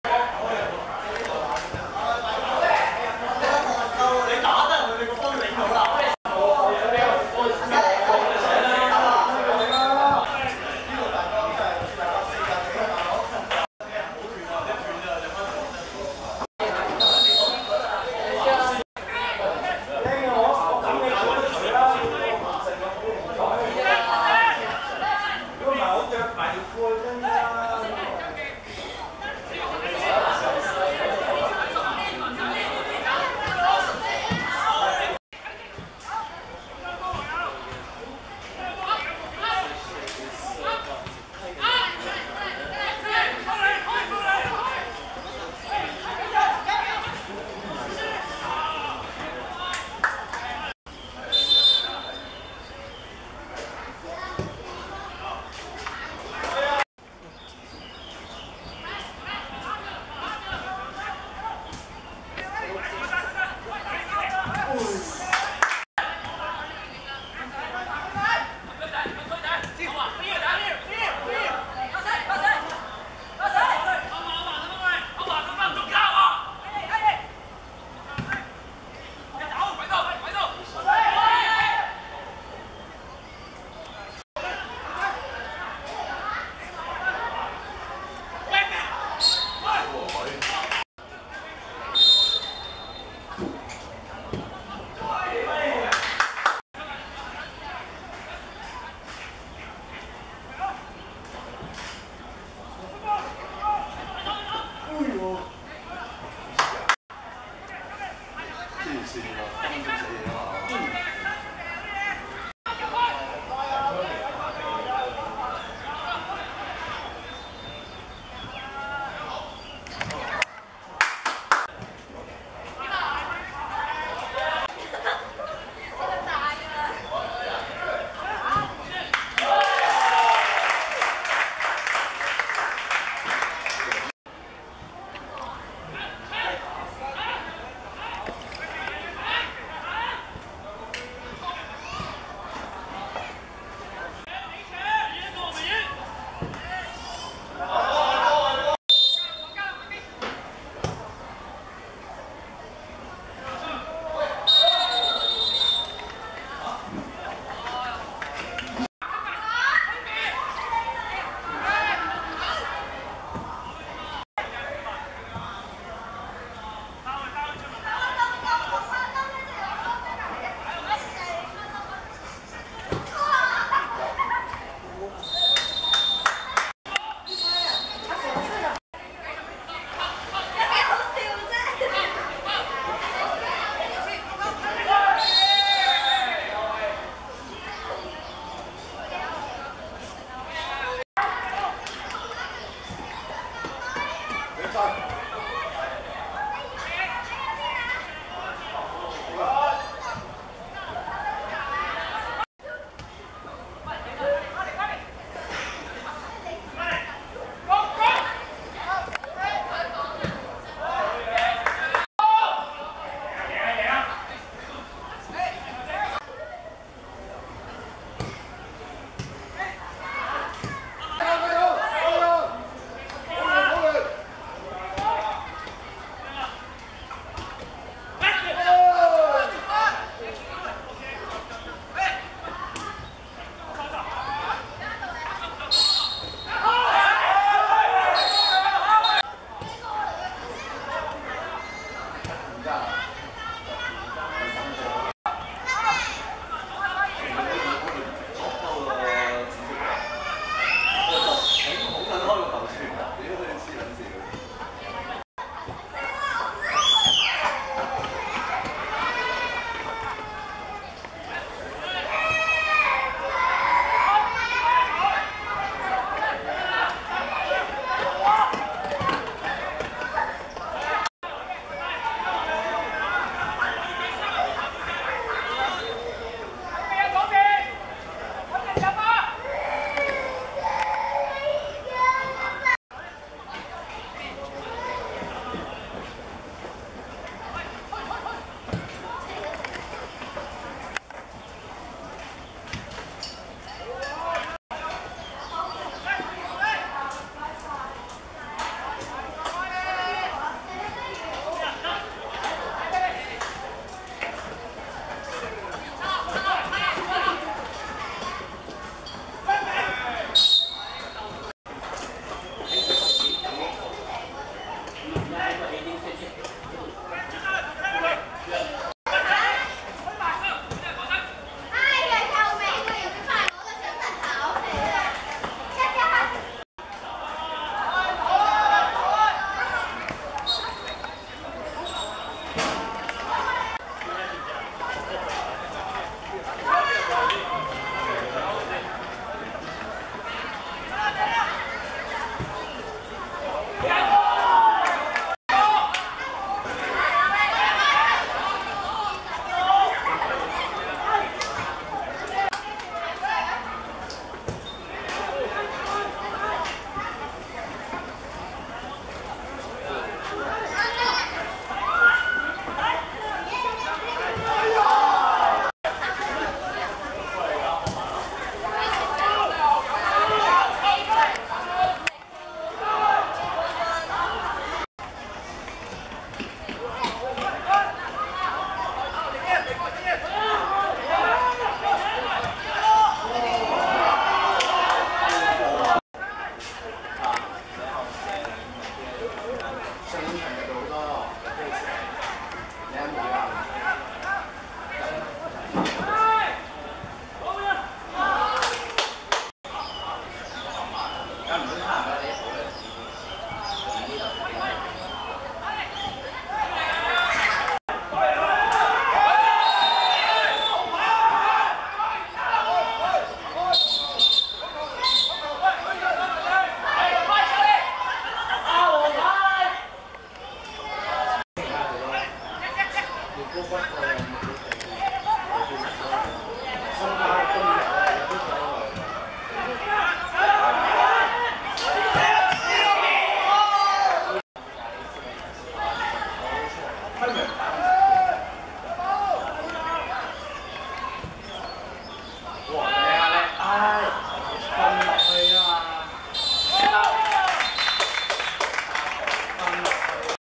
國強vs深水埗(2015.9.20.香港乙組足球聯賽)精華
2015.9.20.九龍仔運動塲 15:30 國強(棕) 0:1 深水埗(藍)